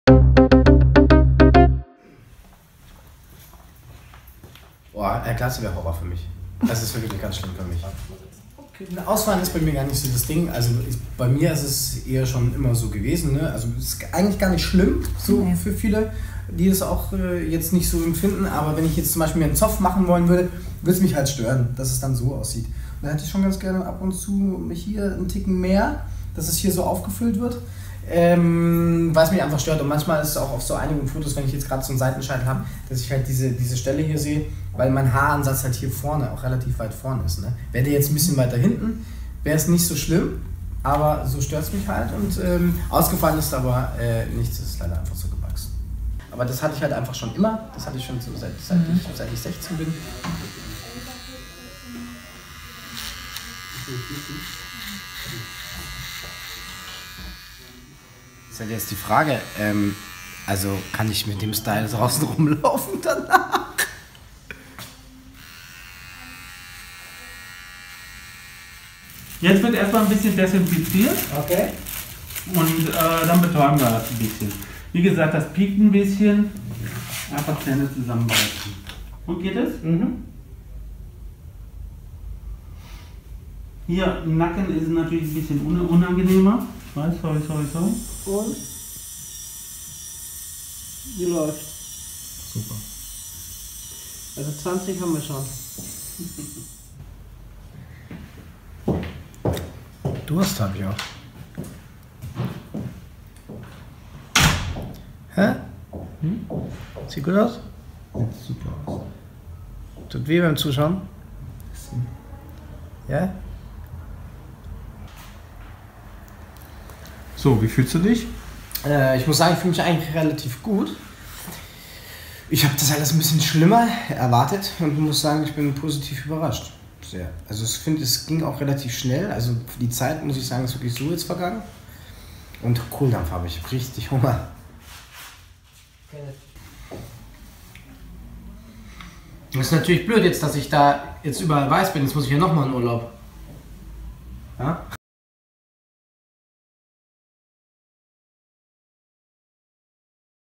Rocco Stark bekommt neue Haare - Haartransplantation
Angst vor der Glatze? Rocco Stark stellt sich einer Haartransplantation. Genau wie seine Ex Kim Gloss.